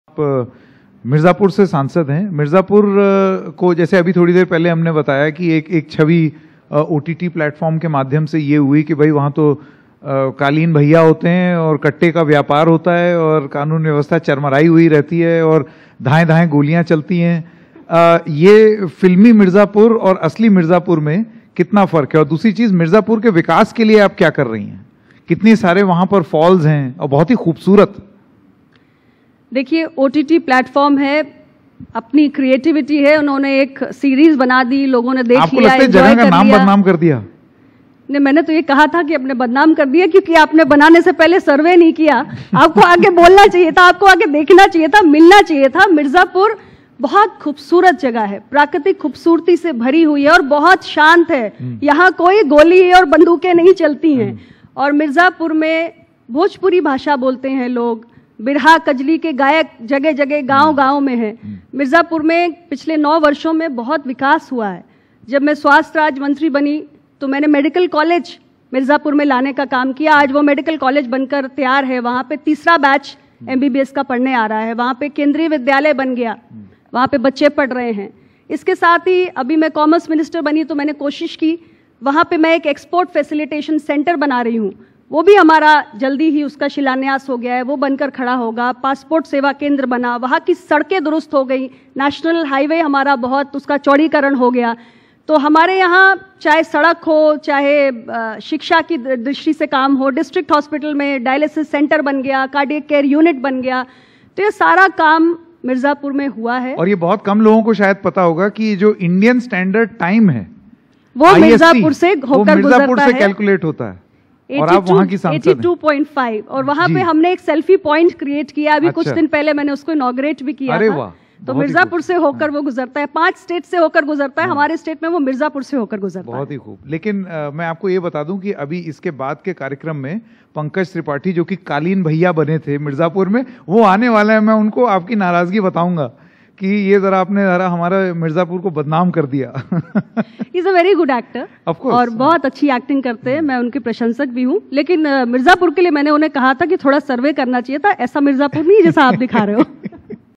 Mirzapur Series से नाम खराब हुआ, Research नहीं की गई : Anupriya Patel
NDTV Conclave में केंद्रीय मंत्री Anupriya Patel ने कहा कि आज का यूपी नया यूपी है. लोगों के सपने पूरे हो रहे हैं. उन्होंने कहा कि मिर्जापुर सीरीज से नाम खराब हुआ. क्योंकि ठीक तरह से रिसर्च नहीं की गई, मिर्जापुर में काफी विकास हुआ है.